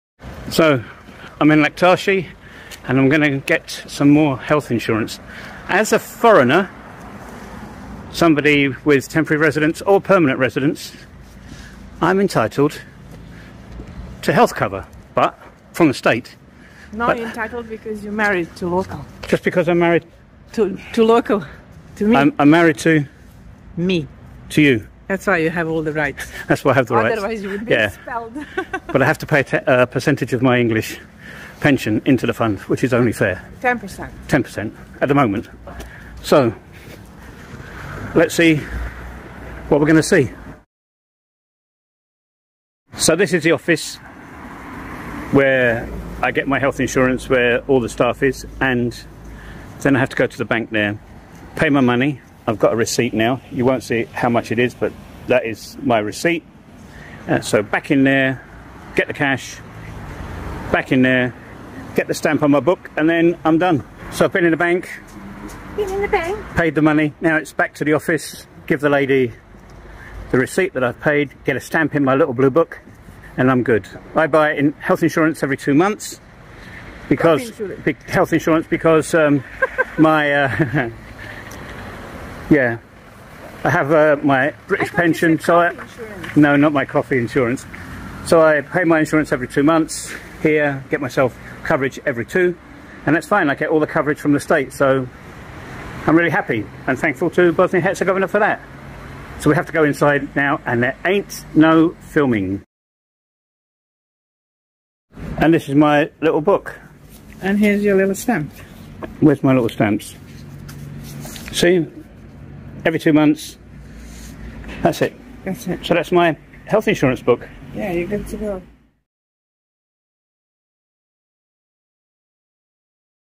State Healthcare in Bosnia
I am able to enjoy the benefits of the local health service.

It's not for free.

As a temporary or permanent resident (not citizen), I can pay 10% of my UK pension into the system.

BTW

My UK pension is NOT as generous as the EU.

In this video, I'm exploring healthcare in Bosnia and Herzegovina. I'm a Brit in the Balkans, and my goal is to show you this amazingly beautiful country.